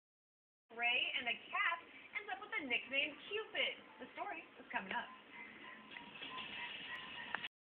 CAT SHOT WITH ARROW
Cat was hit by arrow in freak accident. Not my original video. Owned by some news station.